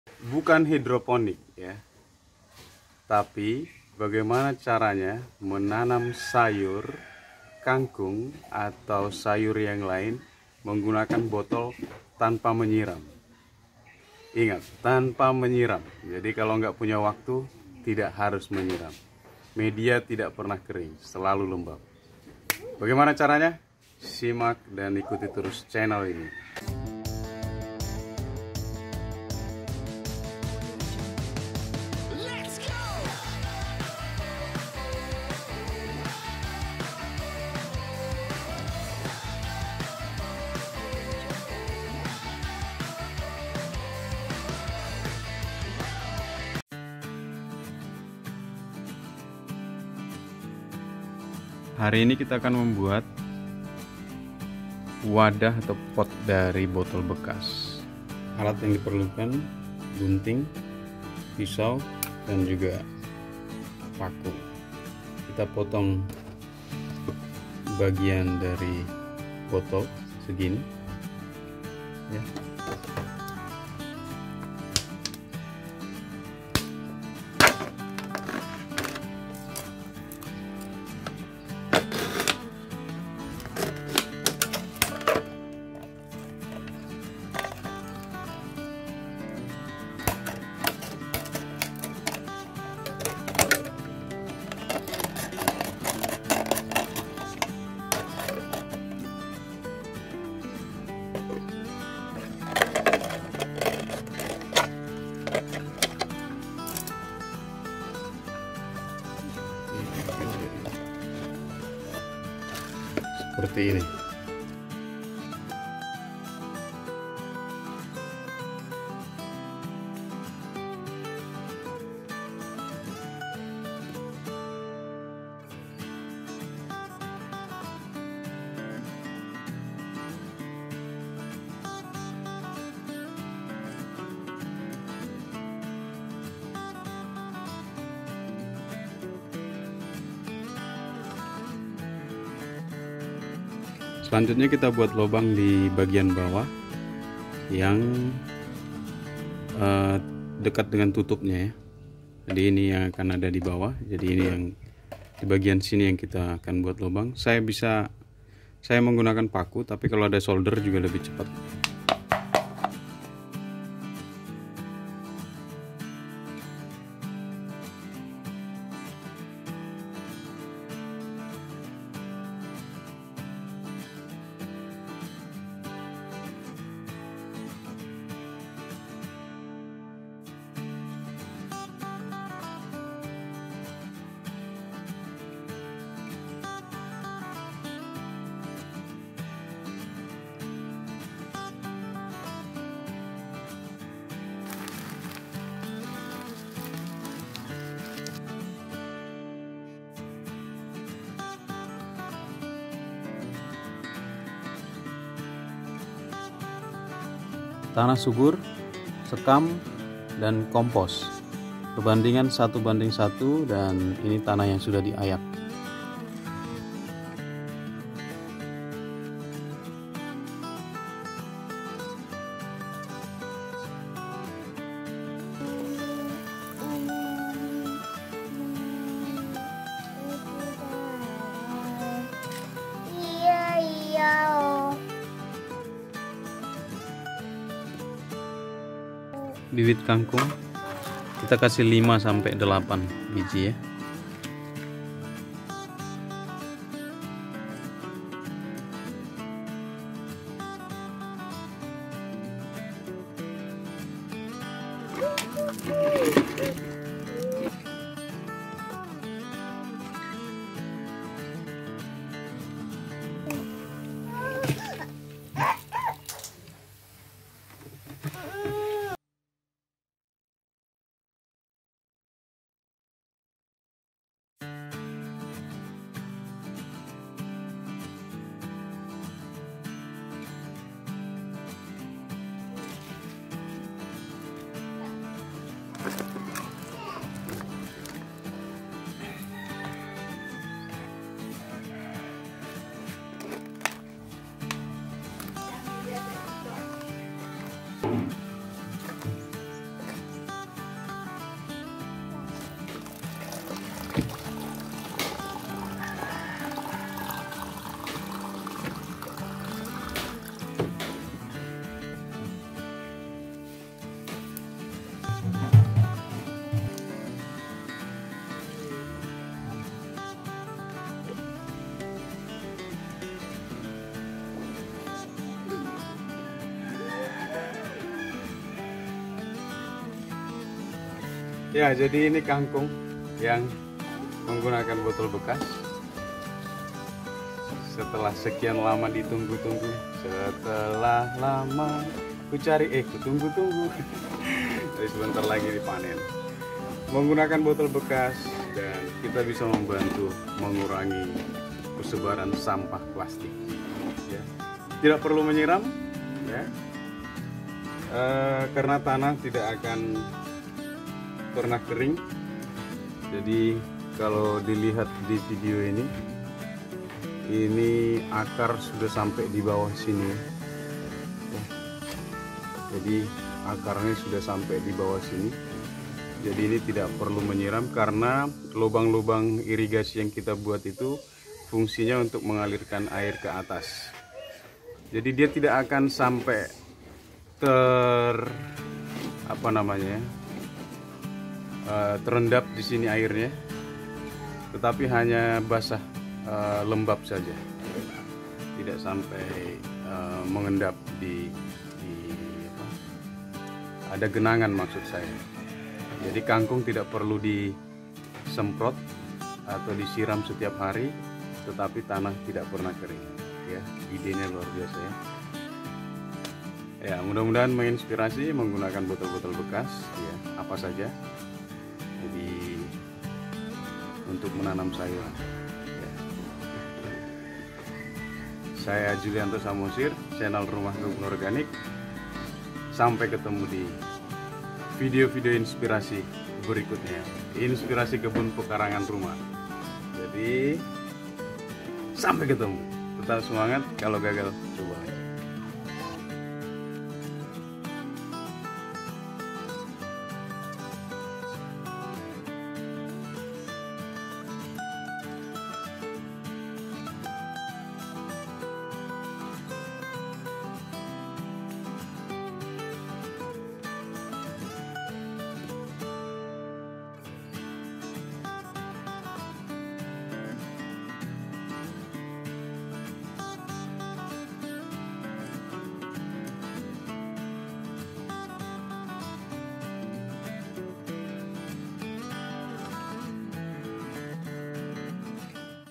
BUKAN HIDROPONIK Menanam Kang Kangkung Di Botol Tanpa Menyiram Sampai Panen Dan AB Mix
Salam
Julianto Samosir